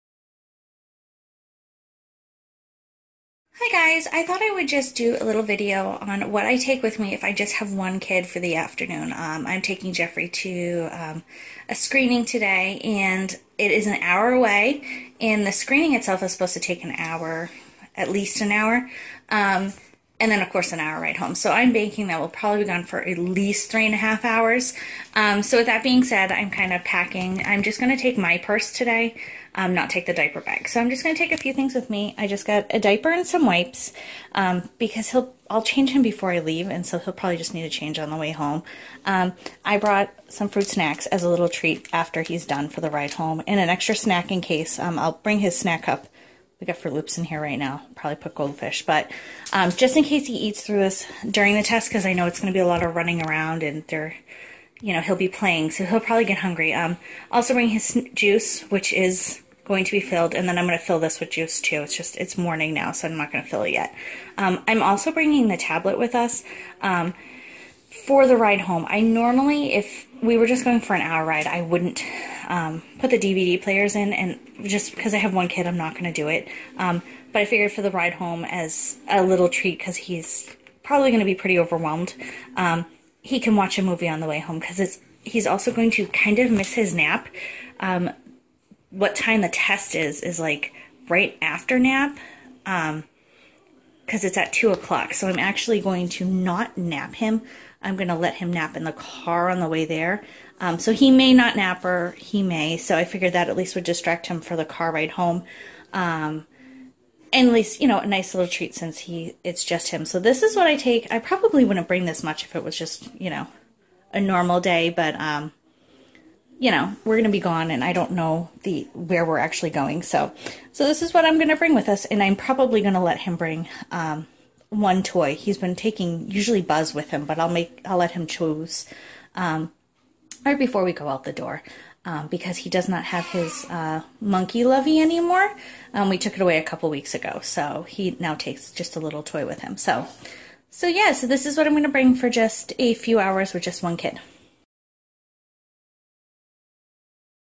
My bag for 1 kid for the afternoon!
I had to take Jeffrey for a test an hour away! Here is what I brought with me:)